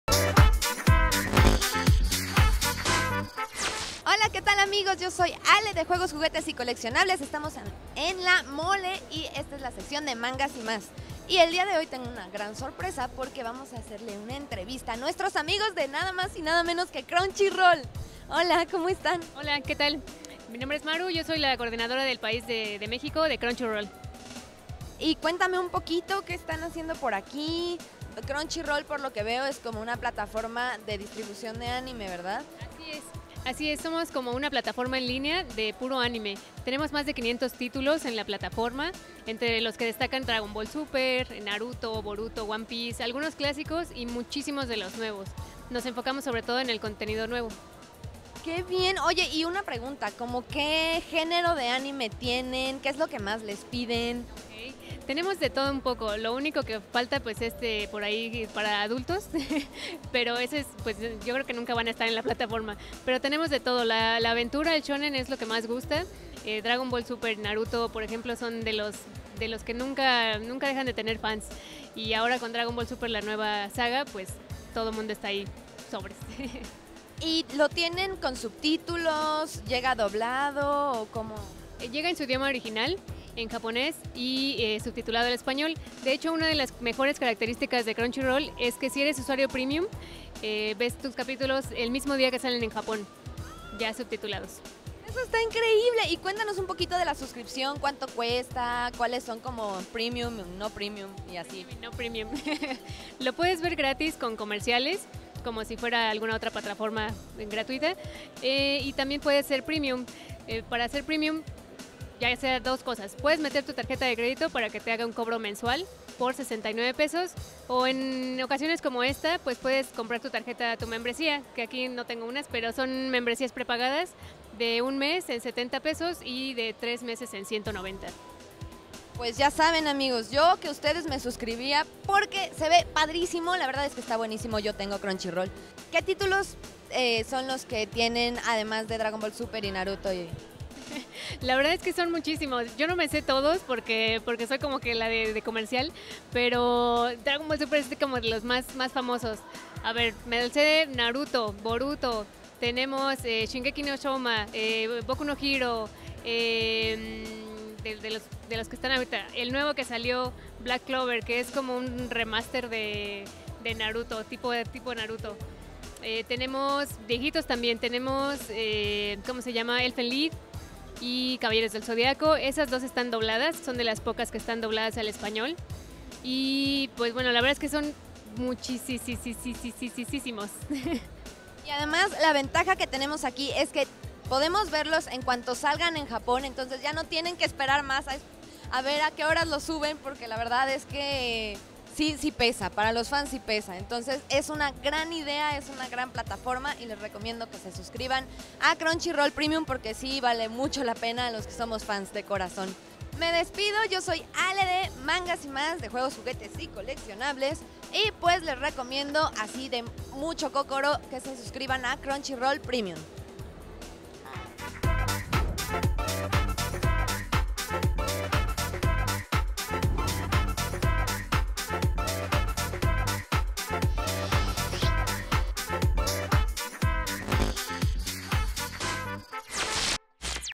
¿Que es Crunchyroll? en La Mole Comic Con Internacional ★ juegos juguetes y coleccionables ★
Ale visita el stand de Crunchyroll en La Mole Comic Con Nov 2017 , conoce los planes, costos y extras que tienen para ti ¿Que es Crunchyroll?